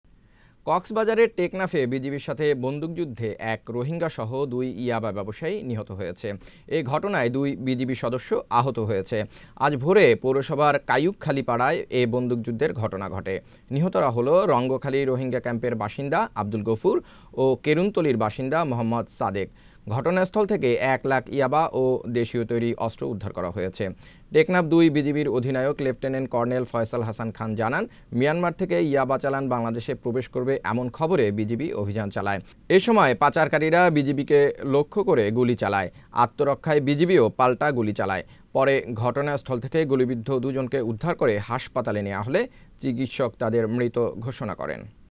টেকনাফে বিজিবির সাথে ‘বন্দুকযুদ্ধে রোহিঙ্গা সহ ২ ইয়াবা ব্যবসায়ী নিহত
কক্সবাজারের টেকনাফে বিজিবির সাথে ‘বন্দুকযুদ্ধে’ এক রোহিঙ্গা সহ ২ ইয়াবা ব্যবসায়ী নিহত হয়েছে। এ ঘটনায় ২ বিজিবি সদস্য আহত হয়েছে।
শনিবার আজ ভোরে পৌরসভার কায়ুকখালী পাড়ায় বন্দুকযুদ্ধের এ ঘটনা ঘটে। নিহতরা হলো রঙ্গিখালী রোহিঙ্গা ক্যাম্পের বাসিন্দা আবদুল গফুর ও কেরুনতলীর বাসিন্দা মোহাম্মদ সাদেক। ঘটনাস্থল থেকে ১ লাখ ইয়াবা ও দেশীয় তৈরী অস্ত্র উদ্ধার করা হয়েছে। টেকনাফ-২ বিজিবির অধিনায়ক লেফটেন্যান্ট কর্নেল ফয়সল হাসান খান জানান, মিয়ানমার থেকে ইয়াবা চালান বাংলাদেশে প্রবেশ করবে এমন খবরে বিজিবি অভিযান চালায়। এ সময় পাচারকারিরা বিজিবিকে লক্ষ্য করে গুলি চালায়। আত্মরক্ষায় বিজিবিও পাল্টা গুলি চালায়। পরে ঘটনাস্থল থেকে গুলিবিদ্ধ দু’জনকে উদ্ধার করে হাসপাতালে নেয়া হলে চিকিৎসক তাদের মৃত ঘোষণা করেন।